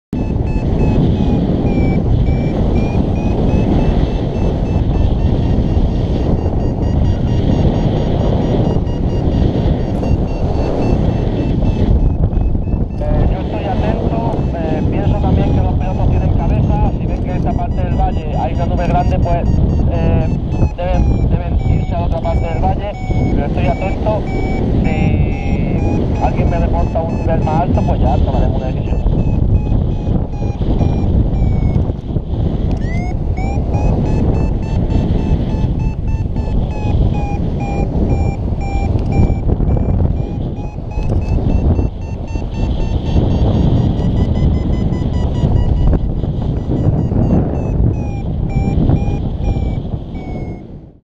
Surfeando la tormenta en parapente
Momento sprevios a la parada de la manga por tormente durante la liga de el Yelmo FIA 2015
2da manga del Open Federacion Liga española en el Yelmo, Jaen durante el FIFA 2015.